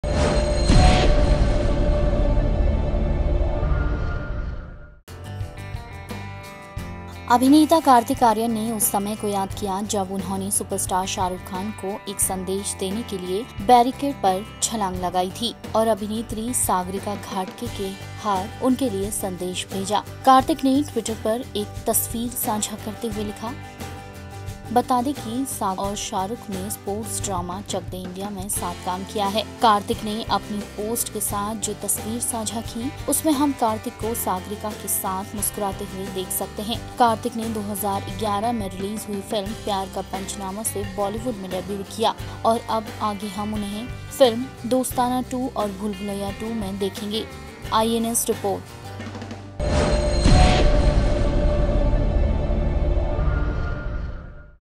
जब Shah Rukh को संदेश देने के लिए Kartik ने बैरिकेड्स पर लगाई थी छलांग
अभिनेता कार्तिक आर्यन ने उस समय को याद किया जब उन्होंने सुपरस्टार शाहरुख खान को एक संदेश देने के लिए बैरिकेड्स पर छलांग लगाई थी और अभिनेत्री सागरिका घाटगे से उनके लिए संदेश भेजा ।